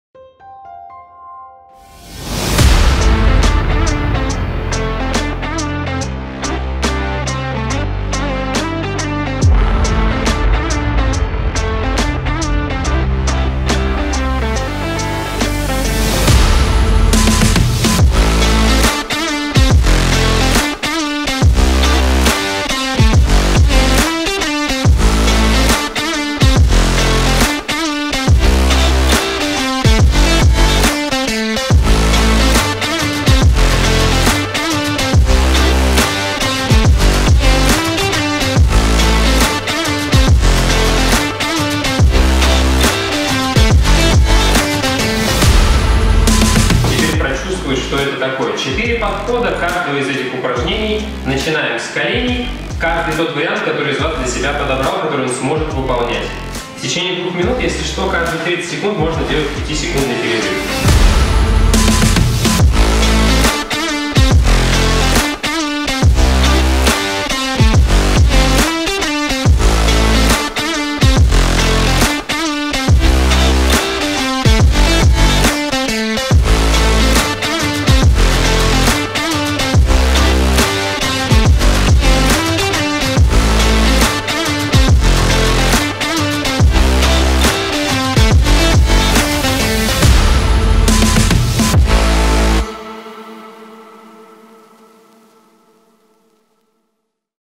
Занятие на подвесных системах в мини группах.
Занятия в мини группах до 4 человек.
Уровень нагрузки адаптируется индивидуально, занятия проводятся как для спортсменов так и людей пришедших для поднятия общей физической формы.